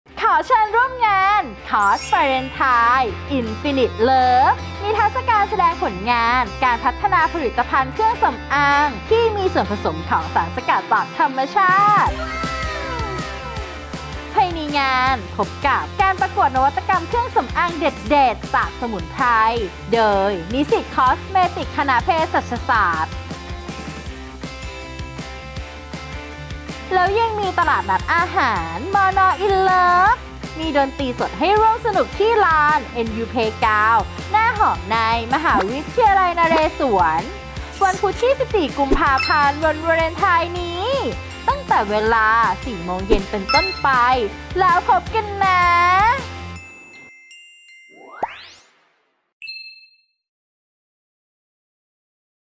คณะเภสัชศาสตร์ ม.นเรศวร ขอเชิญร่วมงาน Cos Fairlentine " Infinite Love " วันวาเลนไทน์นี้ 16.30 น.
ขอเชิญร่วมงาน Cos Fairlentine "Infinite Love" นิทรรศการแสดงผลงานการพัฒนาผลิตภัณฑ์เครื่องสำอางที่มีส่วมผสมของสารสกัดจากธรรมชาติ
ภายในงานพบกับการประกวดนวัตกรรมเครื่องสำอางเด็ดๆ จากสมุนไพร จากนิสิต คอสเมติก คณะเภสัชศาสตร์ แล้วยังมีตลาดนัดอาหาร MorNor in love และดนตรีสดให้ร่วมสนุกๆ กัน ที่ลาน NU Playground หน้าหอใน มหาวิทยาลัยนเรศวร วันพุธที่ 14 กุมภาพันธ์ วันวาเลนไทน์นี้ ตั้งแต่เวลา 16.30 น. เป็นต้นไป แล้วพบกันน้าาาา